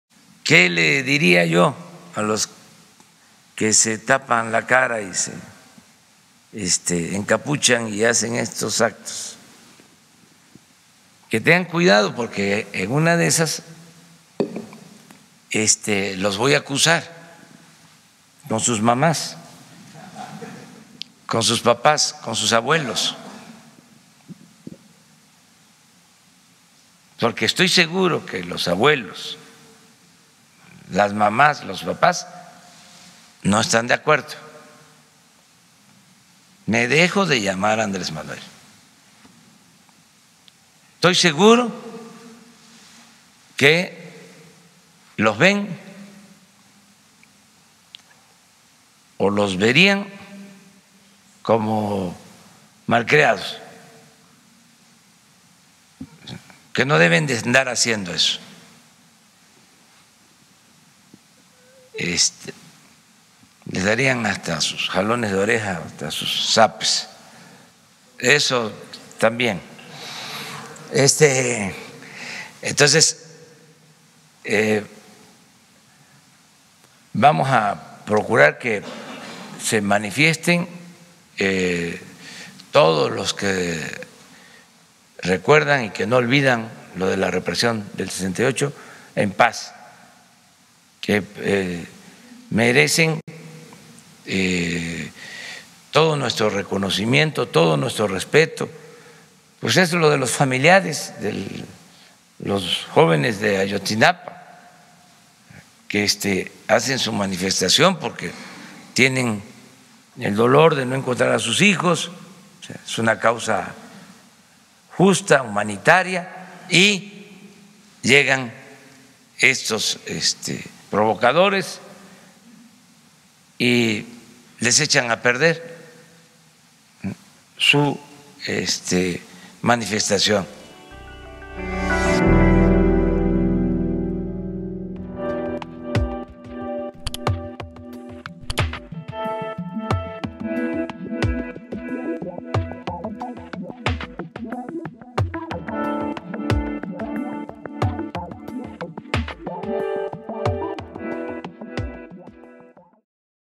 AMLO a encapuchados por vandalismo: 'los voy a acusar con sus mamás'
Al respecto, el mandatario federal expuso que los acusará con sus mamás y sus abuelos.